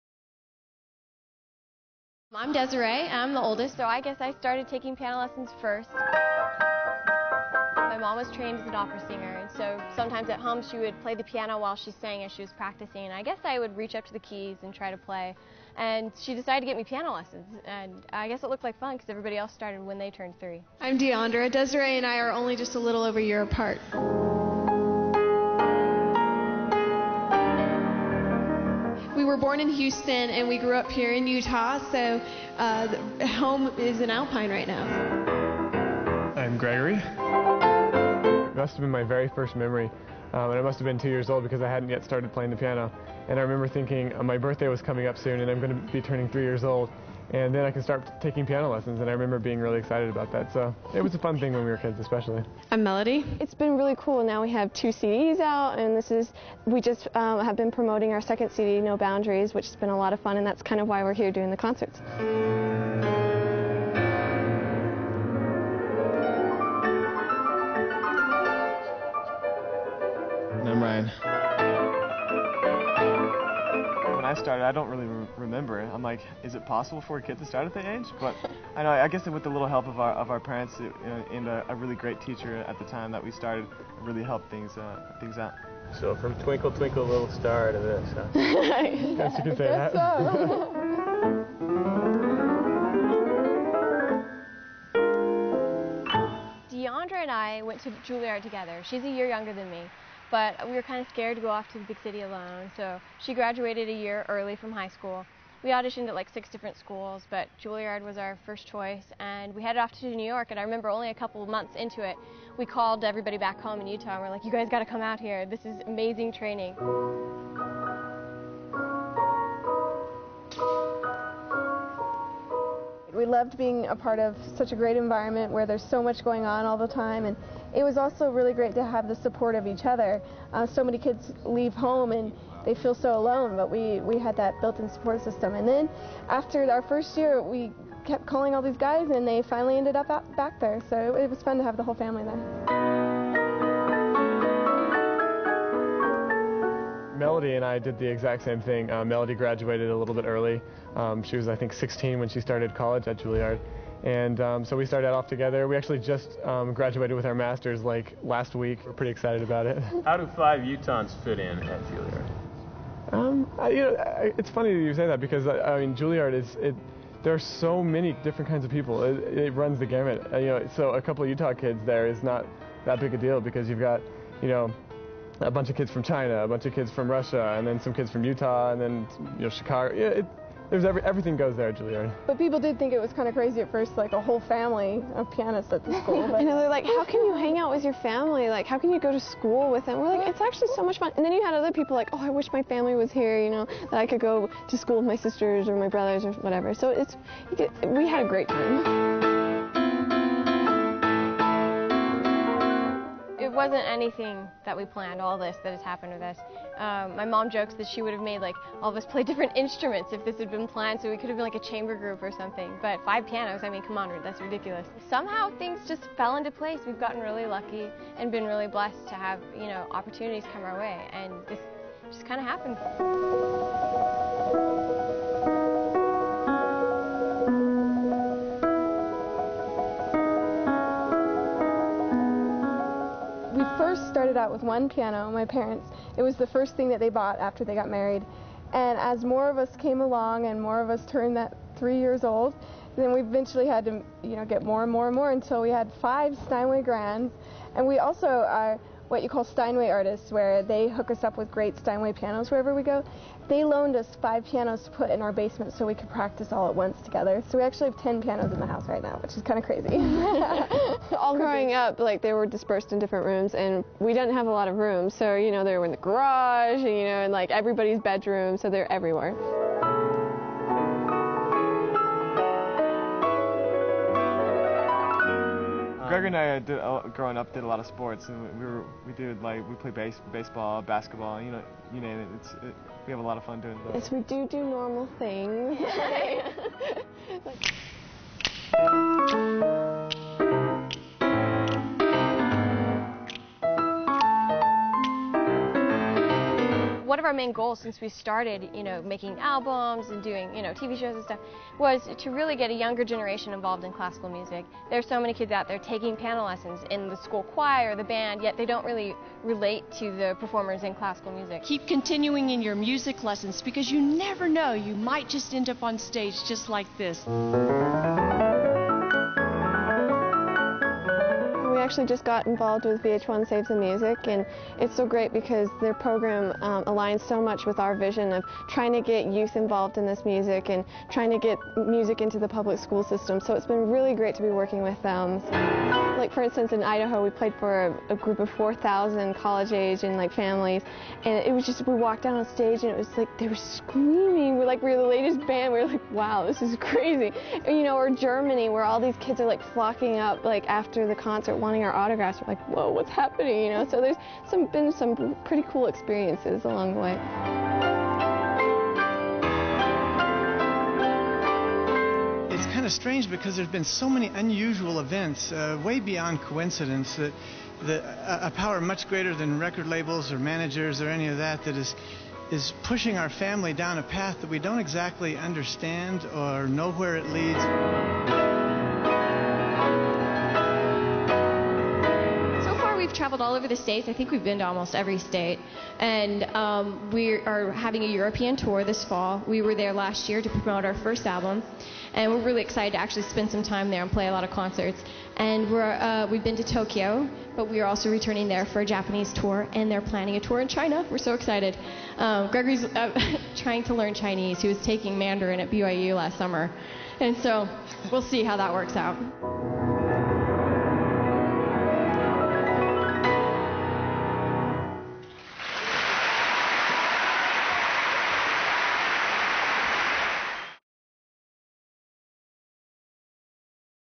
The Five Browns
The piano-playing family talk about how they got their start, and what they're up to next.

For more sports, music & news, be sure to tune in to Park City Television